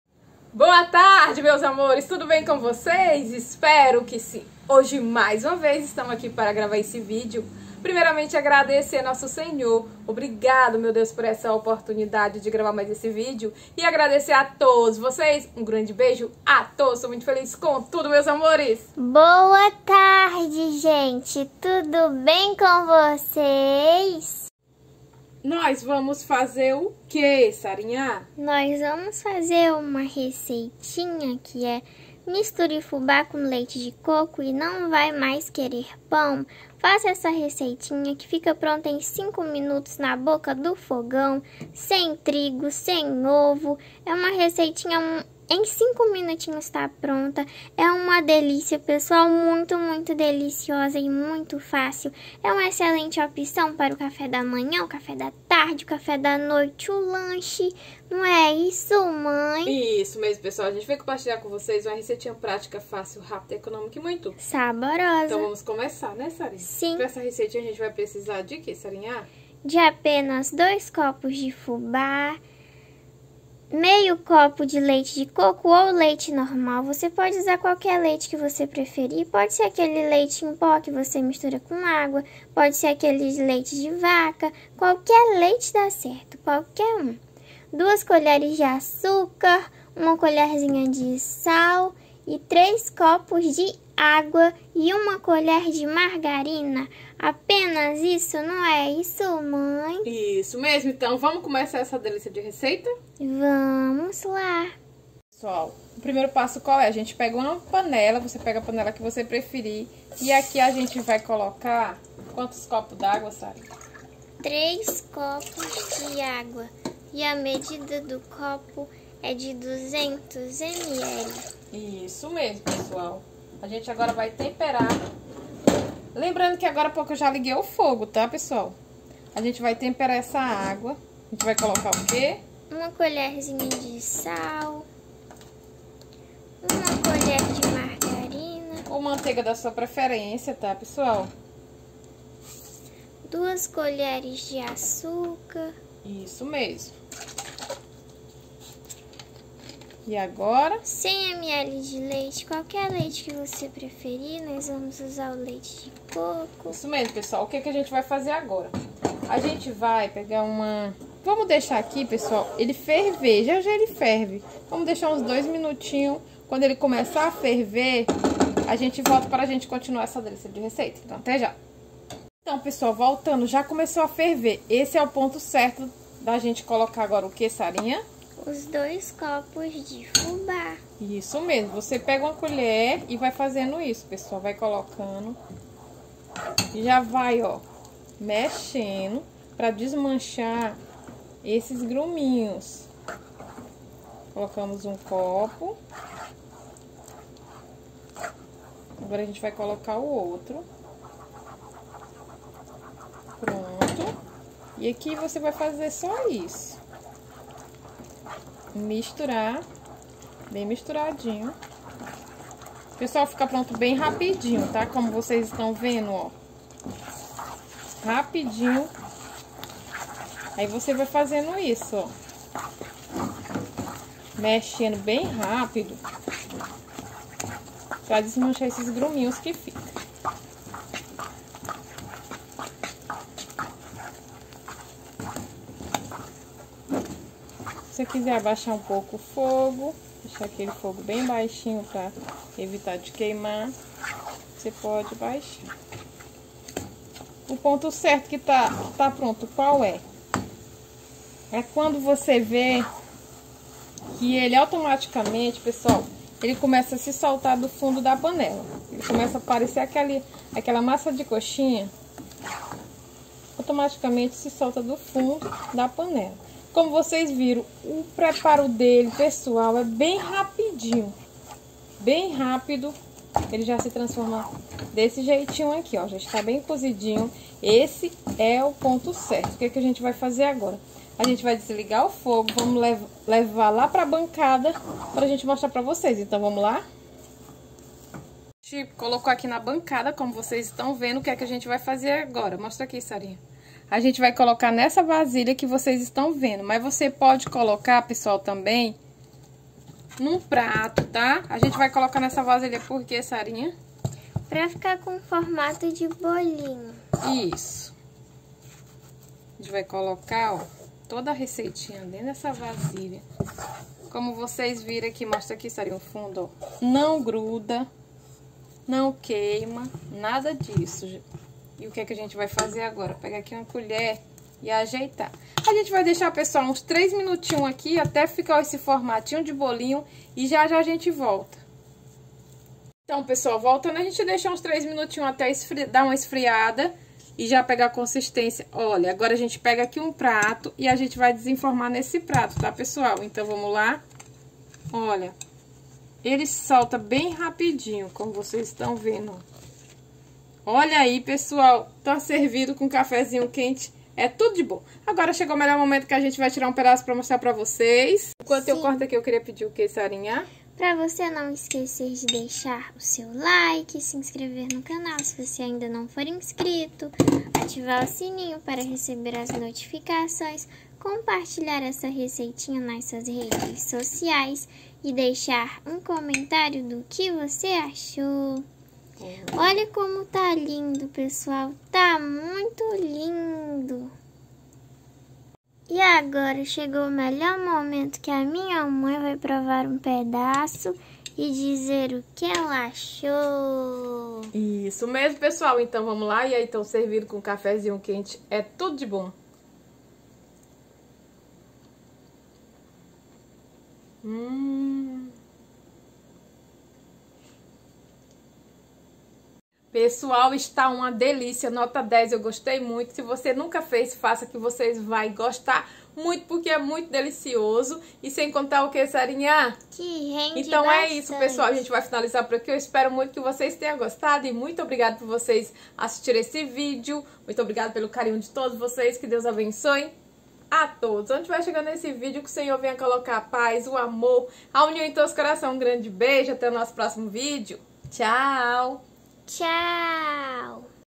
NÃO COMA PÃO,FAÇA ESSE CAFÉ DA MANHÃ SEM TRIGO,SEM OVO PRONTO EM 5 MINUTOS MUITO RÁPIDO E DELICIOSO
ingredientes:
2 copos de fubá
3 copos de água
100ml de leite de coco ou leite normal
1 colher de margarina
2 colheres de açúcar
1 colherzinha de sal